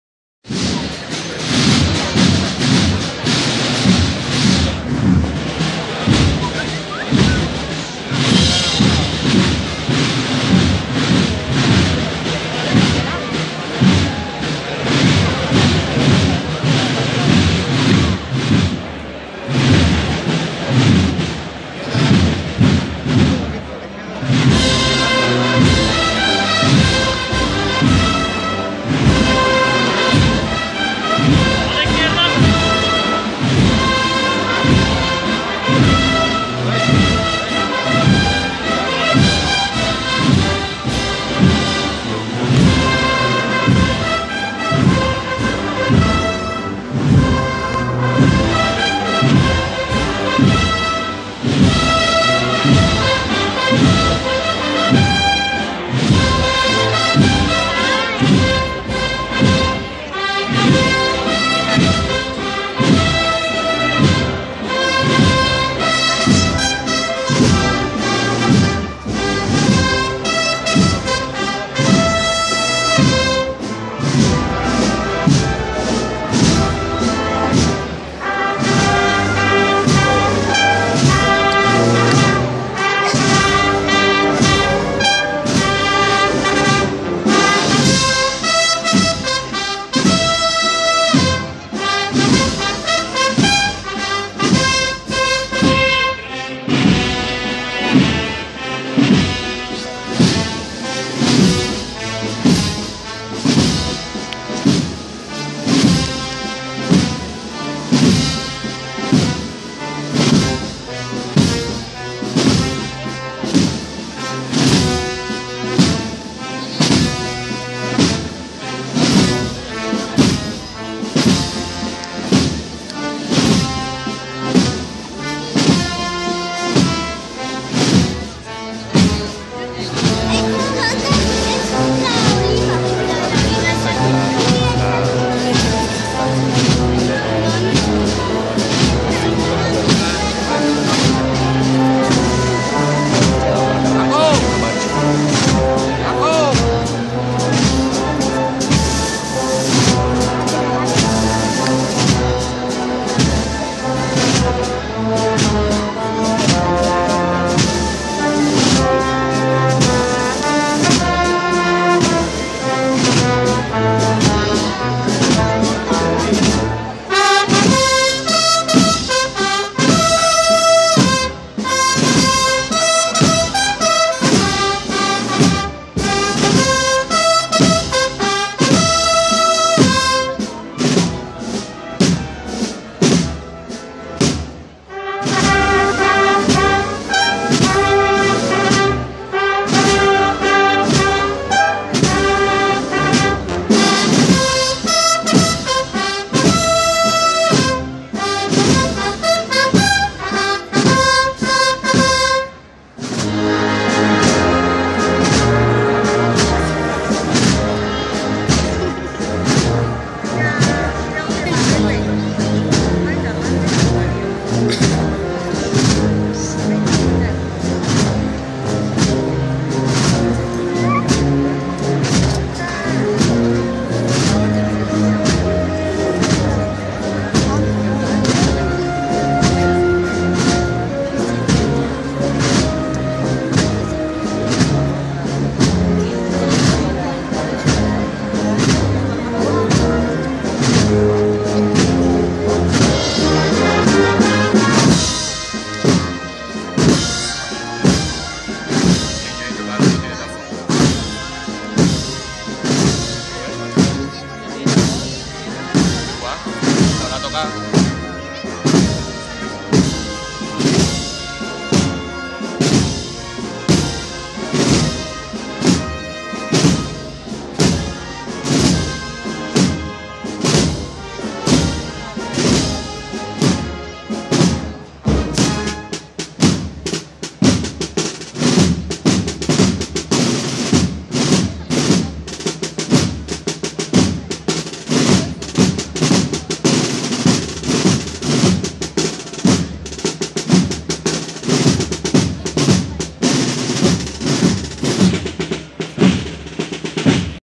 TRASLADO DE LA ESTRELLA 1-3-15 (XV)
Traslado de regreso de Ntro Padre Jesús de las Penas y María Stma de la Estrella tras la celebración del quinario en la Parroquia de Santa Ana.